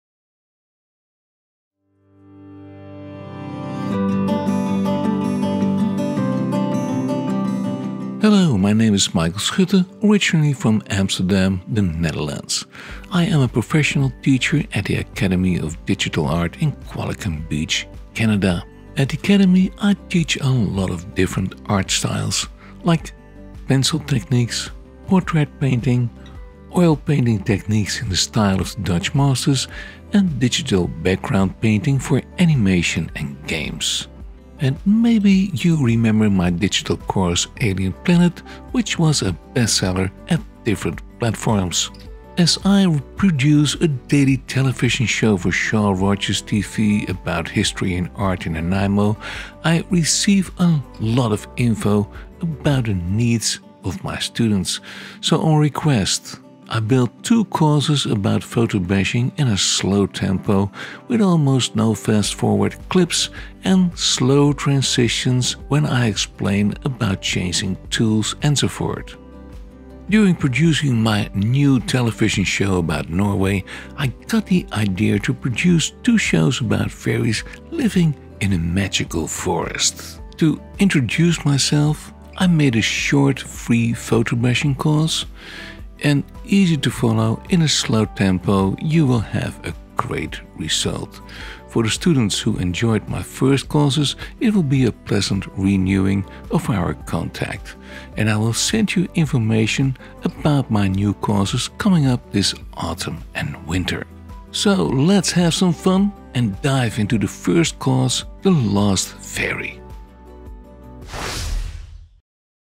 Photobashing and Digital Painting Intro The Lost Fairy Michael Schutte
If you have trouble following tutorials this course might work for you. At a slow pace, you can follow this Photobashing course even if you are a beginner. We will build up a Magical forest with a Fairy and show you the wonderful Techniques of Photobashing and Digital Painting for free.